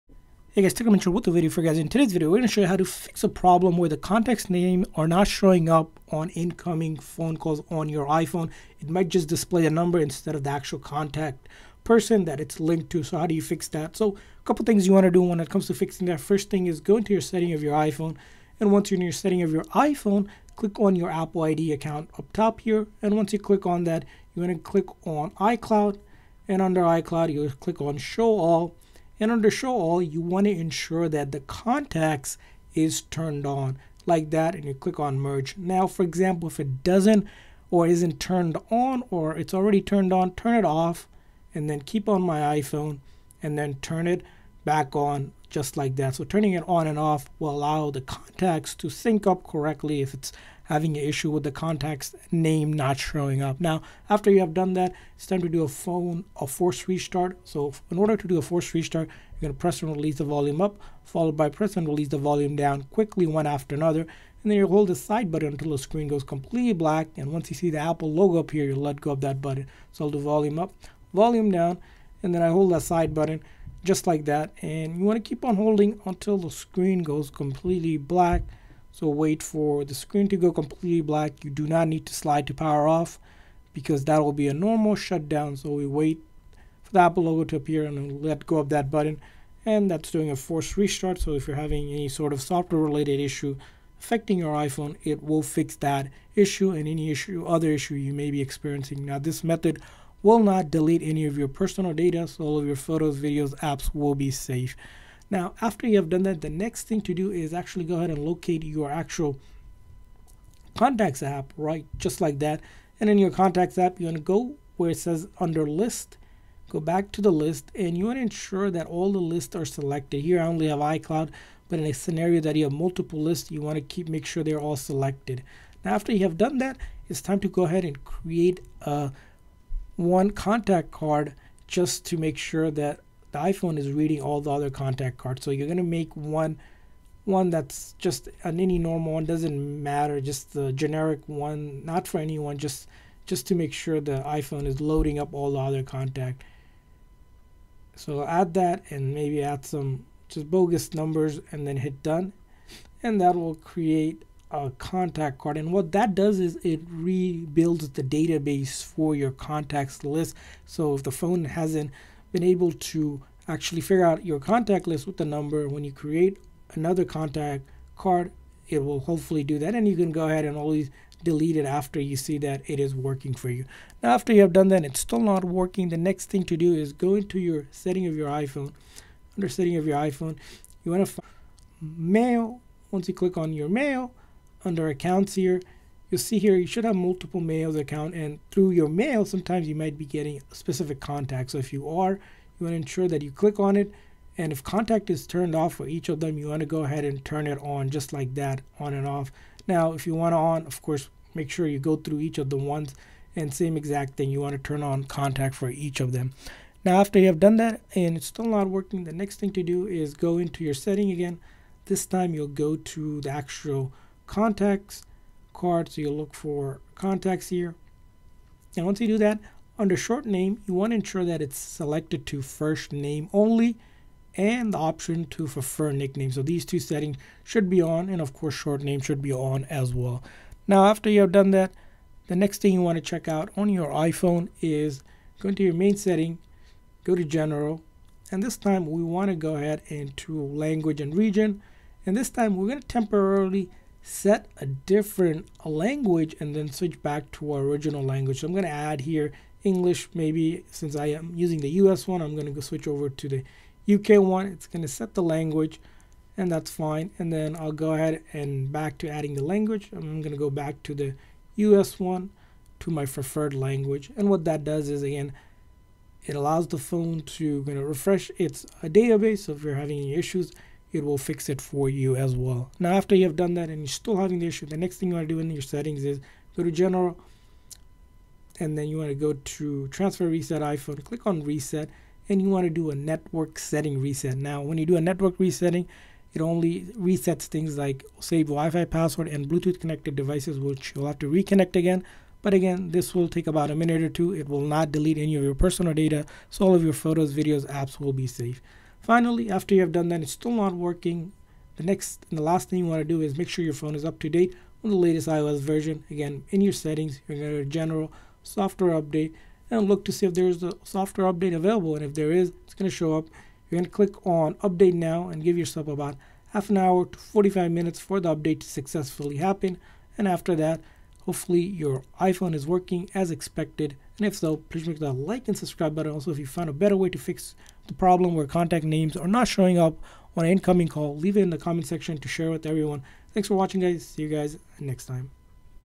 Contact Name Not Showing on Incoming Calls on iPhone (FIXED)
iPhone contact names not showing in incoming calls? In this video we show you how to fix iPhone contact names not showing up on incoming phone calls on iPhone on the latest iOS update.